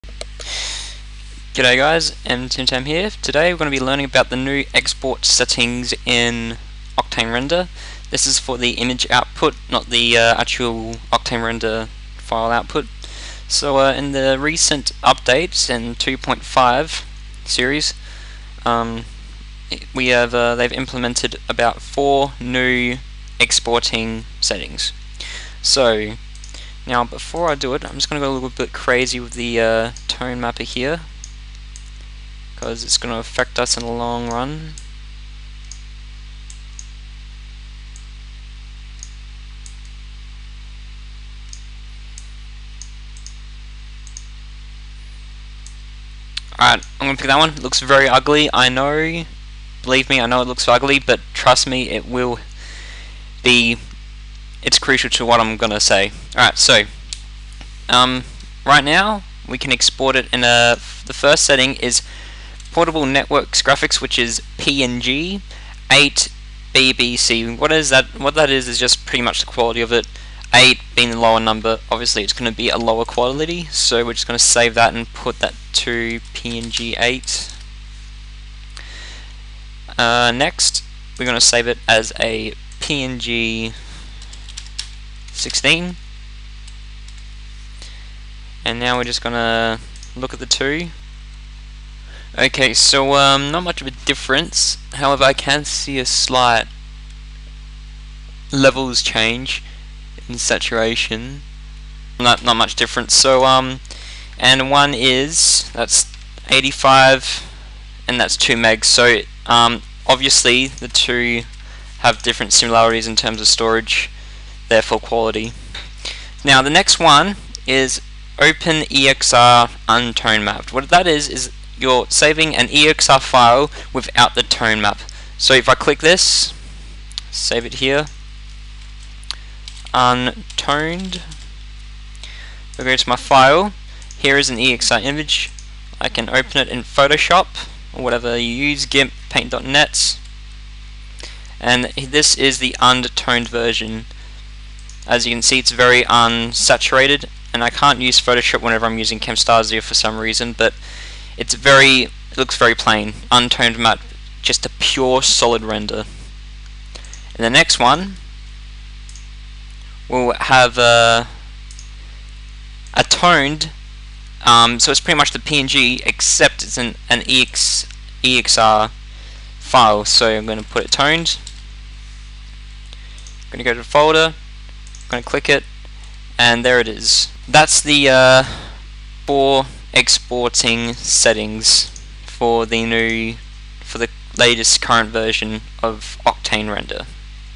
Octane Render Tutorial Series - 14 - New Exporting
In this tutorial we will be going though the new image exporting settings